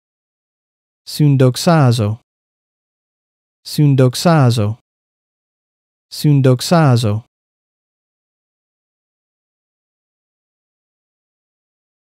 How to pronounce Syndoxazō in Biblical Greek - (συνδοξάζω / glorify together)
συνδοξάζω (syndoxazō) means glorify together in Biblical Greek.

This video shows how to pronounce συνδοξάζω in the Erasmian pronunciation system.

Example Bible Verses:

Romans 8:17
and if children, also heirs—heirs of God and fellow heirs with Christ, if indeed we suffer together with him so that we may also be glorified together with him.